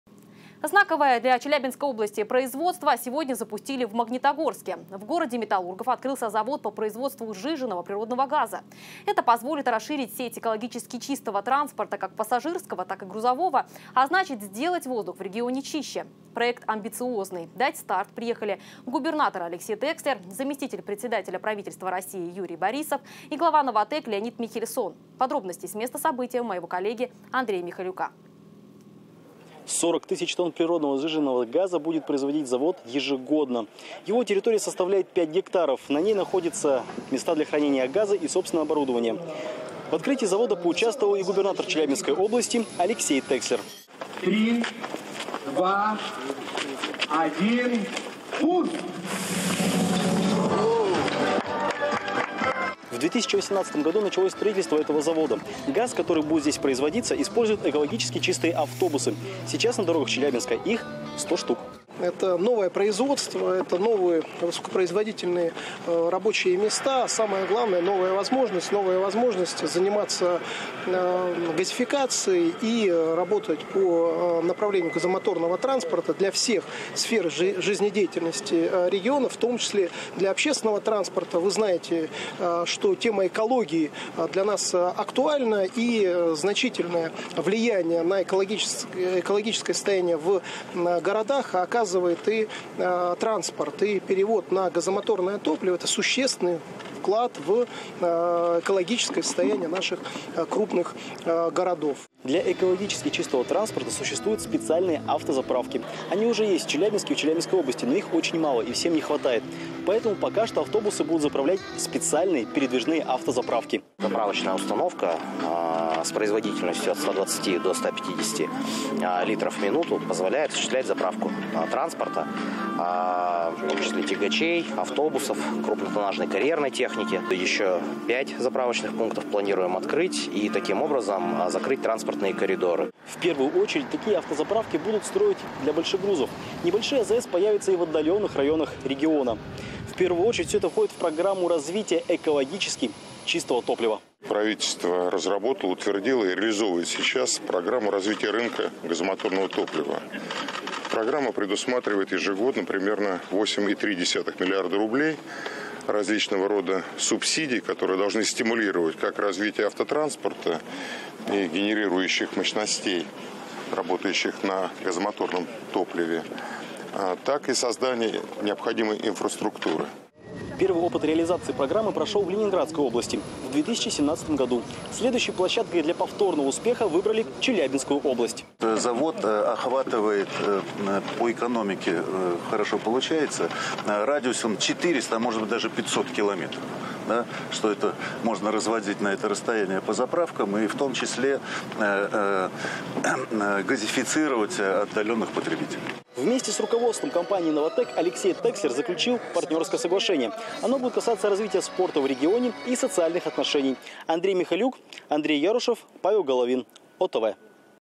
Сюжет «Первого Областного» про запуск малотоннажного СПГ-завода в Магнитогорске
ПАО «НОВАТЭК»  ввело в эксплуатацию первый малотоннажный завод по производству сжиженного природного газа мощностью 40 тыс. тонн в год в Магнитогорске Челябинской области. В рамках политики импортозамещения, которую проводит «НОВАТЭК», завод на 95% оснащен оборудованием отечественного производства.
Продукция завода будет в основном использоваться в качестве газомоторного топлива. Планируются поставки СПГ на заправочные комплексы «НОВАТЭКа» для пассажирского, грузового транспорта и карьерной техники в Челябинской области и в соседних регионах.
В церемонии запуска приняли участие заместитель председателя правительства РФ Юрий Борисов, замглавы Минэнерго РФ Павел Сорокин, председатель правления ПАО «НОВАТЭК» Леонид Михельсон, губернатор Челябинской области Алексей Текслер.